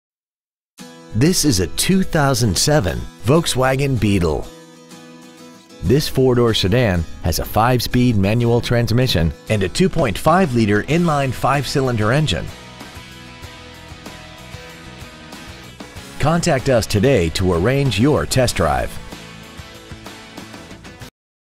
2007 Volkswagen Beetle Puyallup WA
Year : 2007
 Make : Volkswagen
 Model : Beetle
 Engine : 2.5L 5Cyl
 Trans . : 5 Spd Manual
 Exterior : Blue
 Miles : 80,510
 Interior : Black
 Stock : V7766

 Used 2007 Volkswagen Beetle Puyallup WA